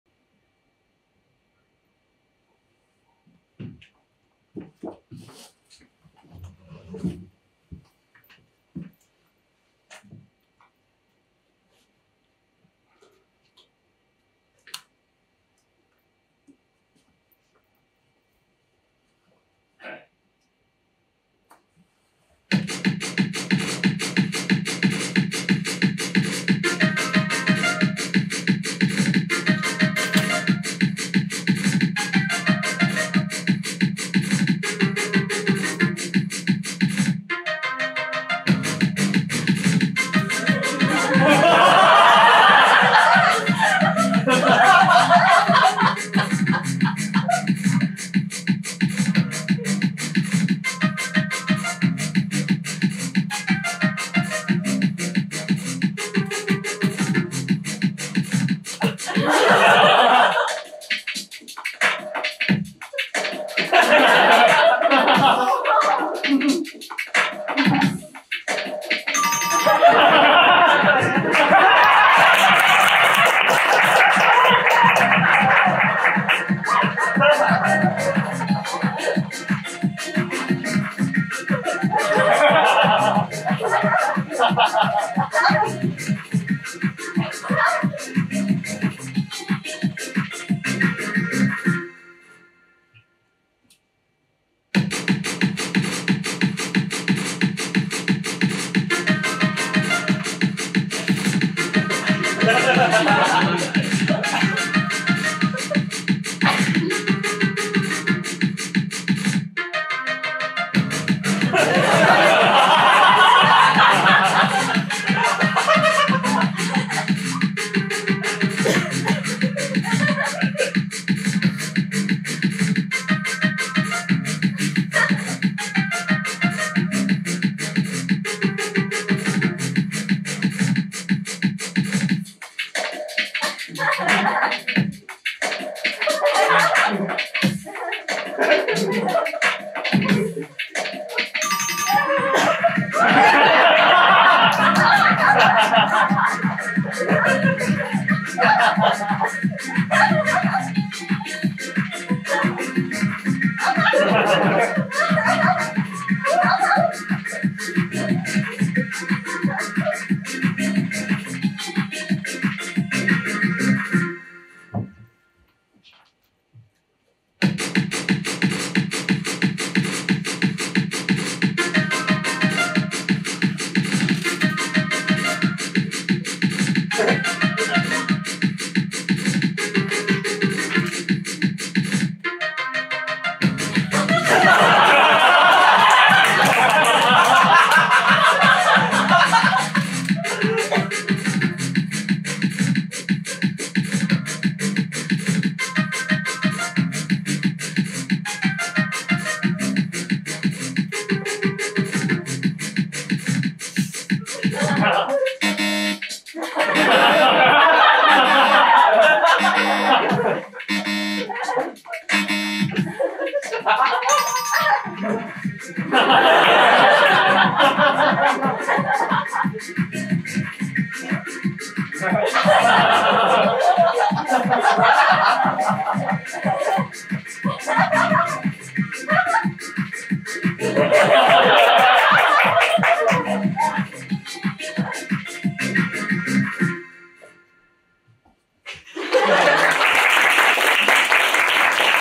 【ピンネタ】きょうの健康【FAN】【8月22日の彼女】
2023/11/26
FAN×蛇口捻流ピンネタツーマンライブ「デッサンビーム」にて披露

🎶単独ライブで制作したオリジナル楽曲を各種サイトで配信中🎶
🔴AppleMusic

🎁8月22日の彼女公式通販「ハチカノ屋さん」